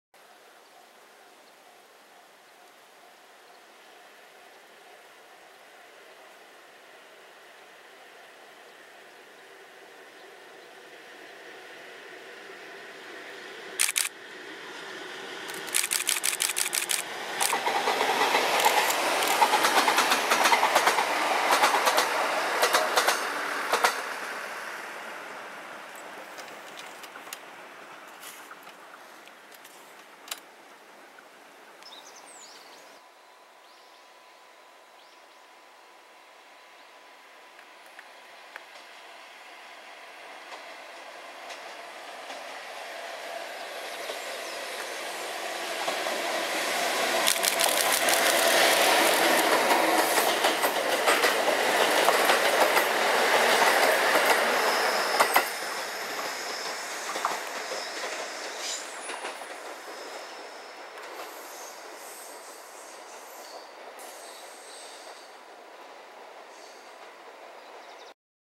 2016年6月30日 奥羽本線・E193系 キヤ甲種輸送
北海道検測を終え、本州へと帰ってきたE193系・East i-D
秋田貨物駅までは、EF510-17号機牽引の甲種輸送でした